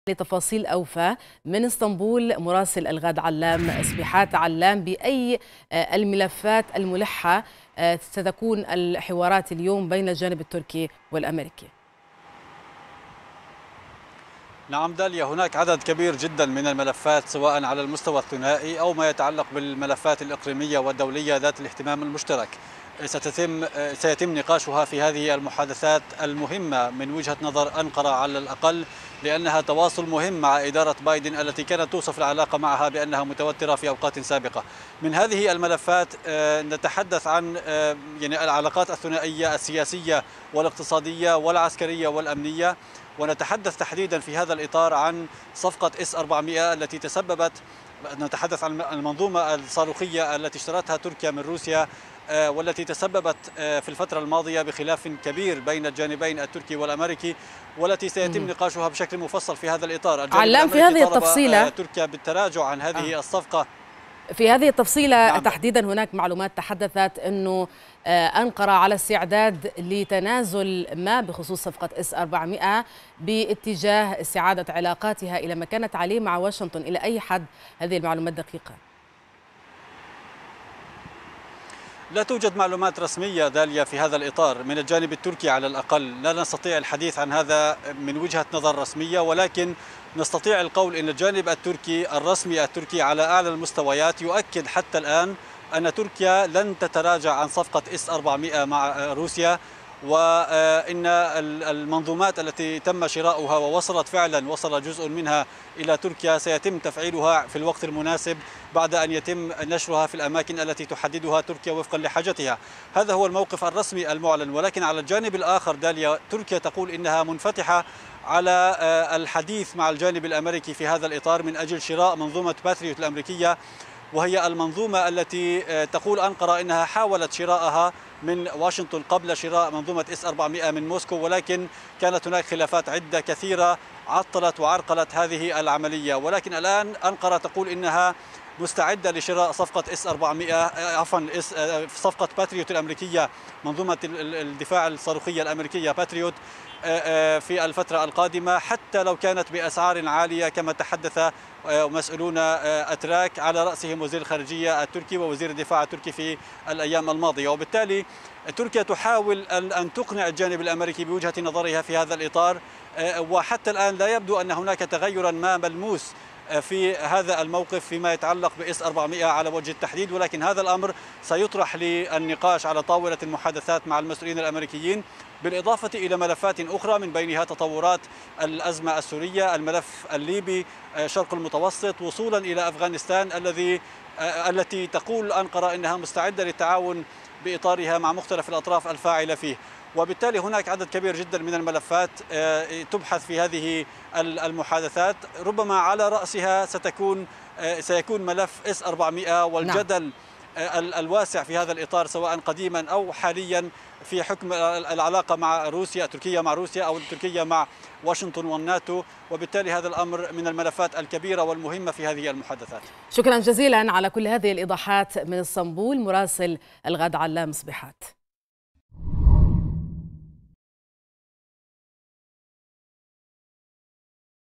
مشاورات تركية أمريكية.. هل أنقرة ستقدم تنازلات في صفقة إس 400؟
يبدأ وفد تركي برئاسة نائب وزير الخارجية زيارة إلى واشنطن تستمر يومين يناقش خلالها مع المسئولين الأمريكيين العلاقات الثنائية والقضايا الإقليمية وخاصة الوضع في سوريا و العراق.
المزيد من التفاصيل يتحدث، من اسطنبول علام صبيحات مراسل الغد، عبر النشرة الإخبارية.